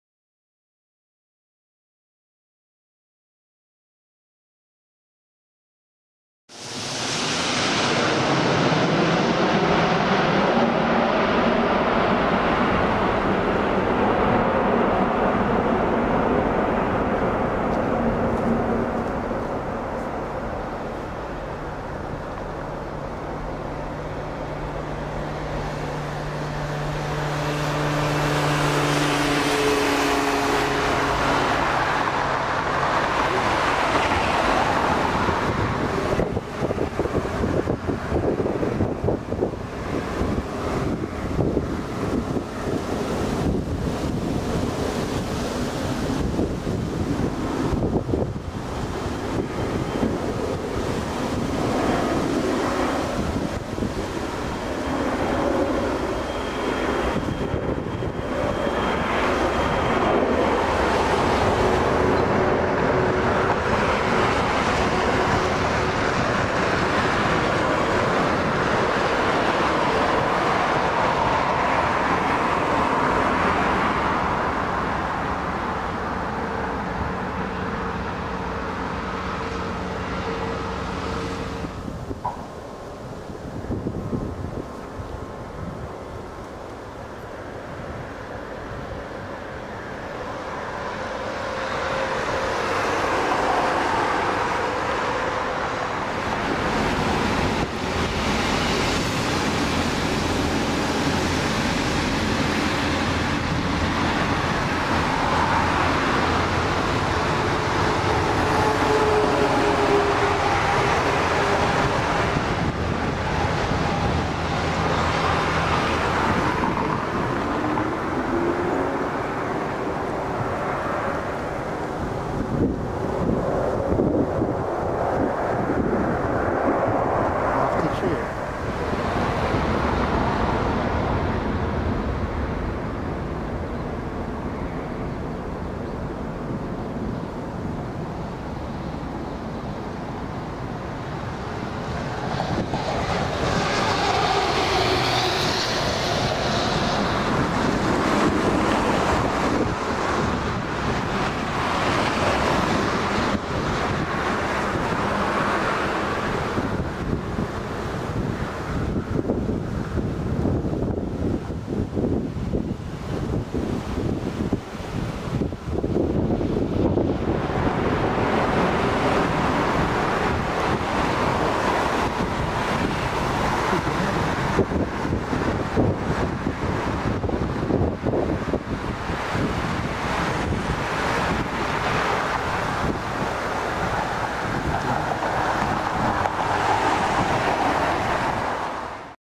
Frankfurt Airport | Landings with light crosswind
Landings of several 'heavies' at Frankfurt Airport with light crosswind